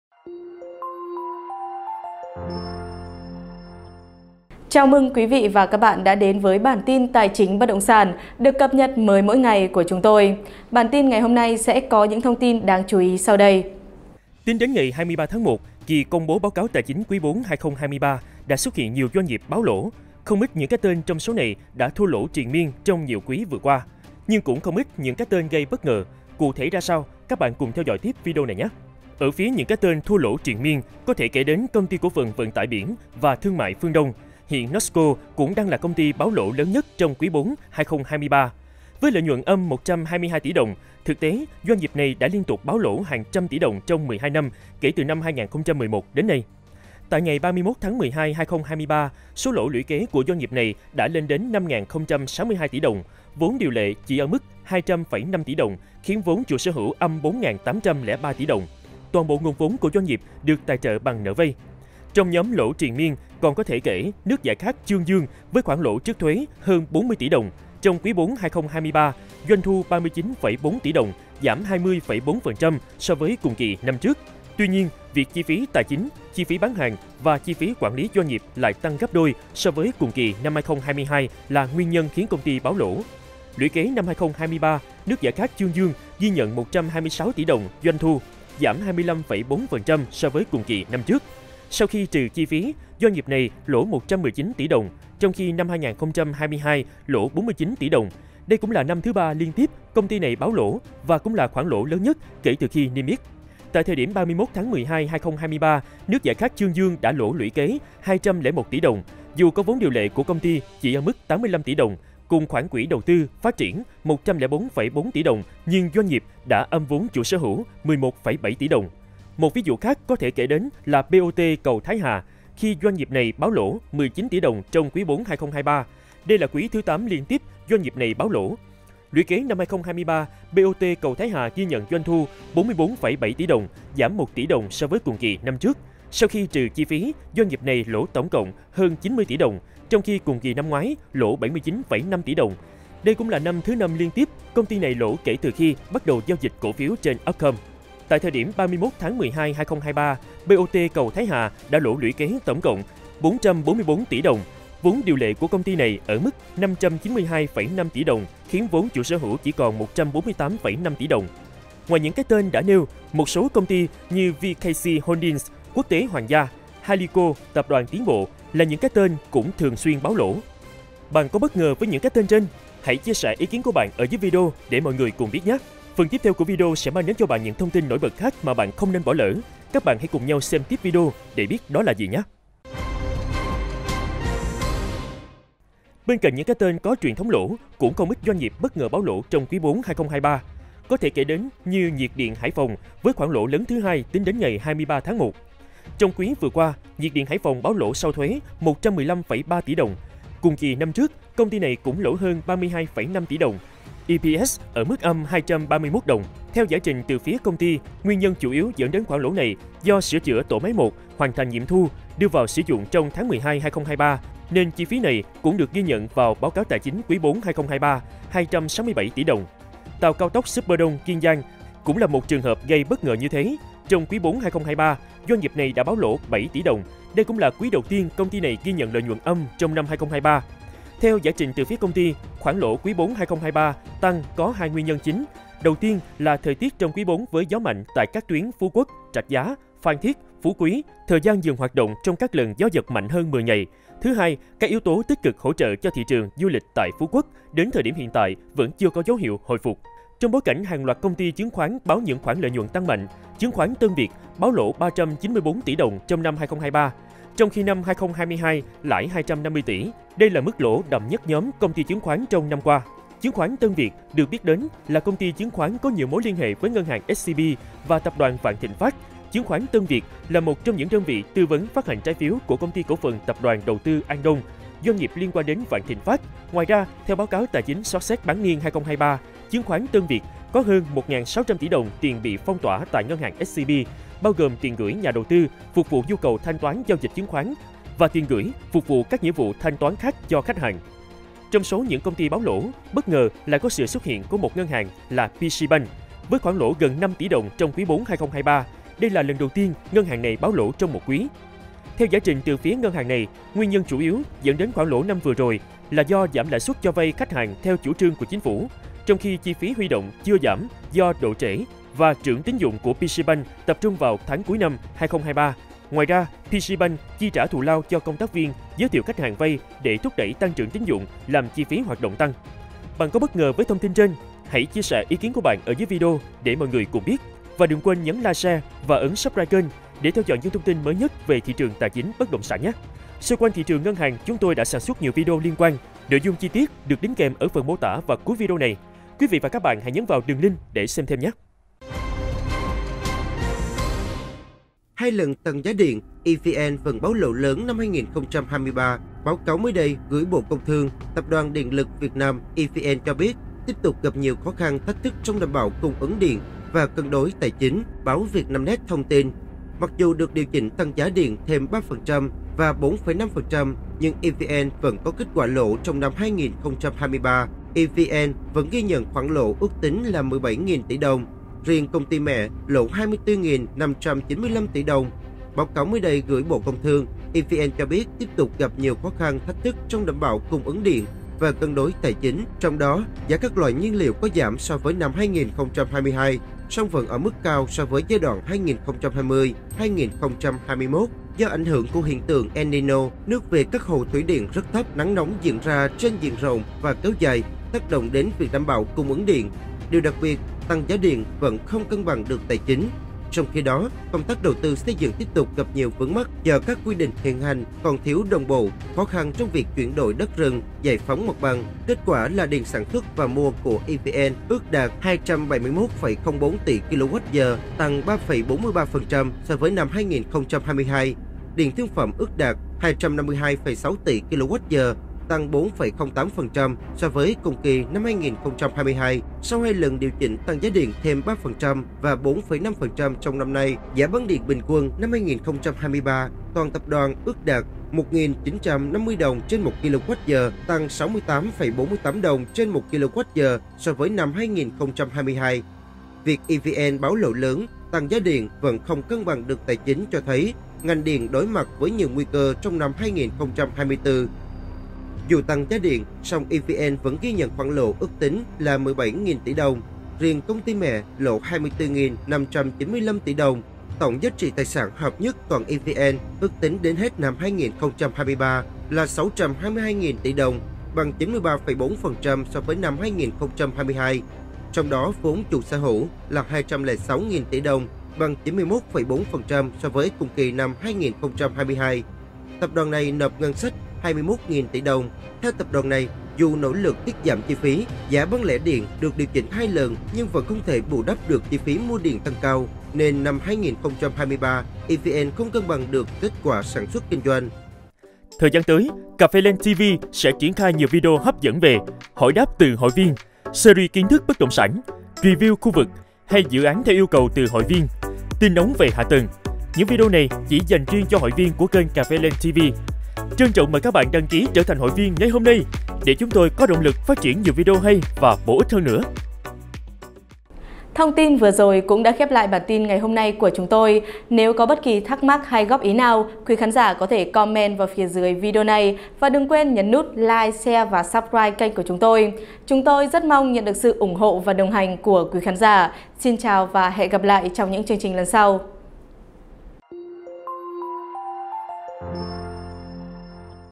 Những khoản lỗ "ĐAU ĐỚN" và bất ngờ, có cả ngân hàng và 1 công ty liên quan Vạn Thịnh Phát| CafeLand
Những khoản lỗ "ĐAU ĐỚN" và bất ngờ, có cả ngân hàng và một công ty liên quan đến Vạn Thịnh Phát

Tính đến ngày 23/1, kỳ công bố Báo cáo tài chính quý 4/2023 đã xuất hiện nhiều doanh nghiệp báo lỗ. Không ít những cái tên trong số này đã thua lỗ triền miền trong nhiều quý vừa qua, nhưng cũng không ít những cái tên gây bất ngờ. Cụ thể ra sao, các bạn cùng theo dõi tiêp video này nhé.
Ở phía những cái tên thua lỗ triền miên có thể kể đến CTCP Vận tải biển và Thương mại Phương Đông. Hiện Nosco cũng đang là công ty báo lỗ lớn nhất trong quý 4/2023 với lợi nhuận âm 122 tỷ đồng. Thực tế, doanh nghiệp này đã liên tục báo lỗ hàng trăm tỷ đồng trong 12 năm kể từ năm 2011 đến nay.